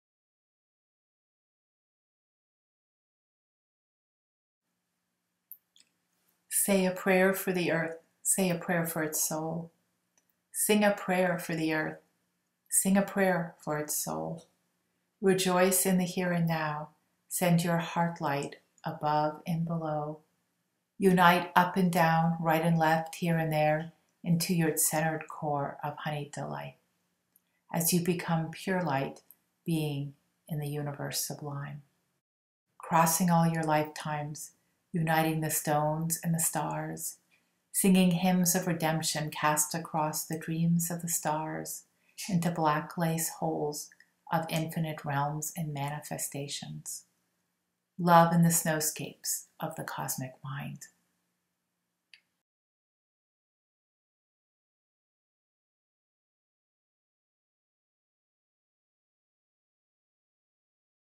Prayer For The Earth, by Skye W. Stephenson
Thinking of the Earth and Earth Day coming up soon!  Feeling like the Earth would really appreciate our prayers and gratitude right now! Here is one small offering to her.